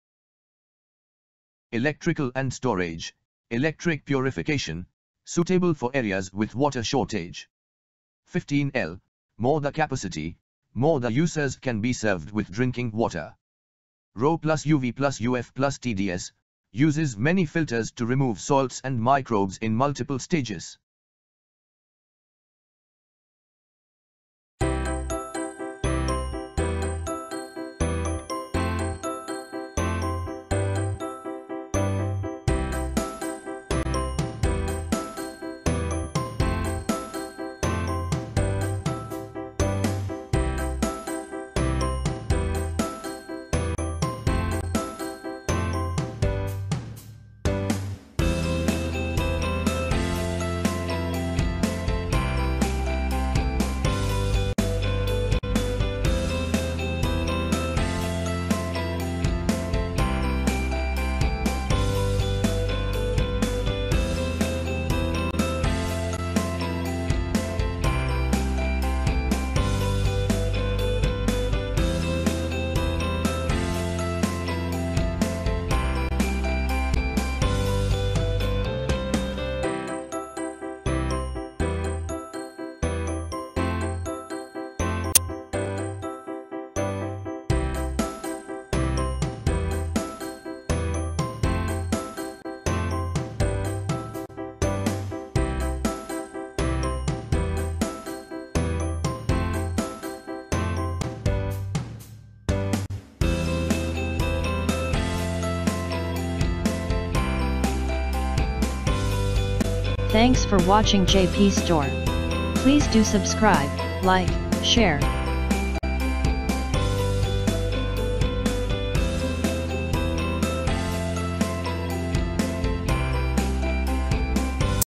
Aqua Fresh NYC MINERAL 15 L ro+uv+uf+tds+Mineral 15 L RO + UV + UF + TDS Water Purifier White
Electrical & Storage : Electric purification - suitable for areas with water shortage
15 L : More the capacity, more the users can be served with drinking water
RO + UV + UF + TDS : Uses many filters to remove salts & microbes in multiple stages